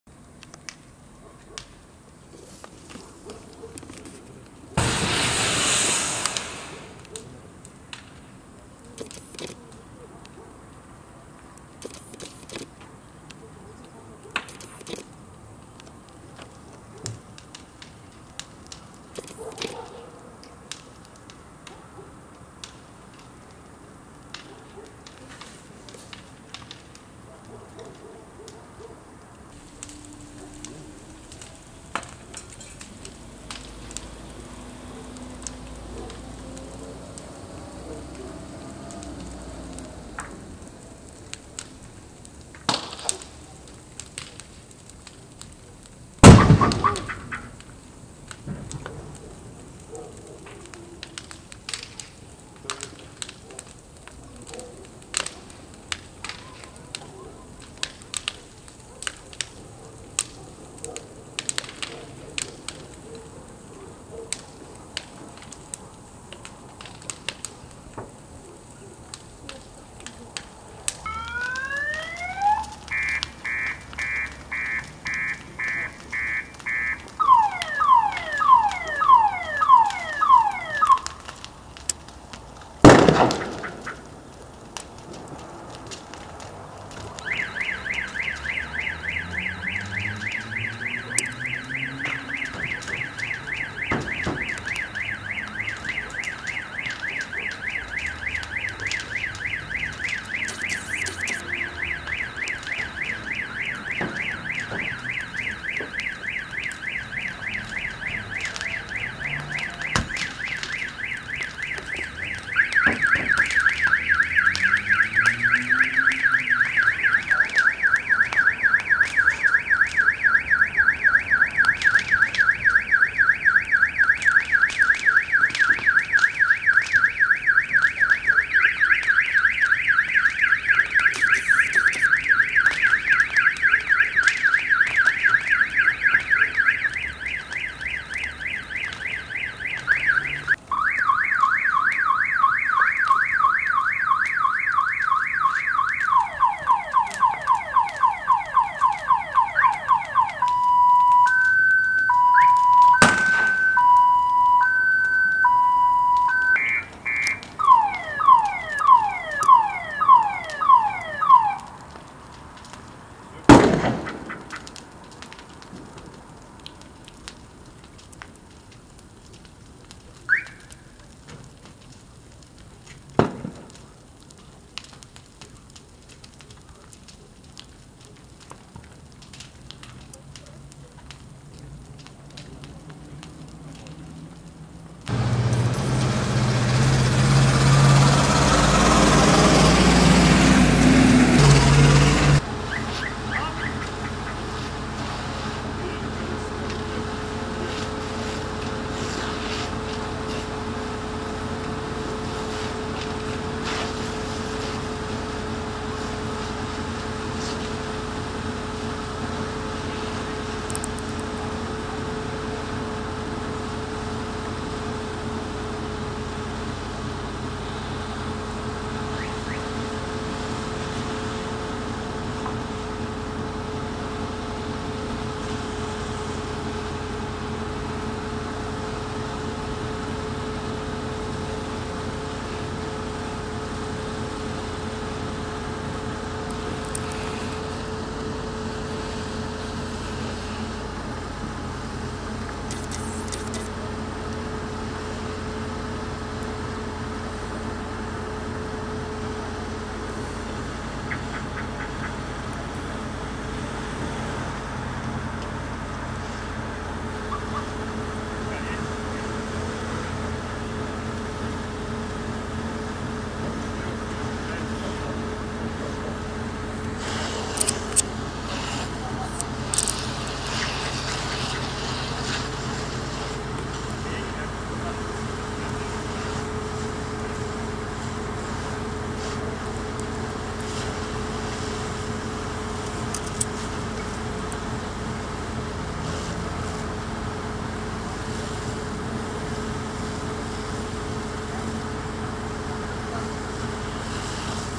Горит Porsche Cayenne burning car
Возгорание произошло около часа ночи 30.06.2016 на парковке во дворе жилого дома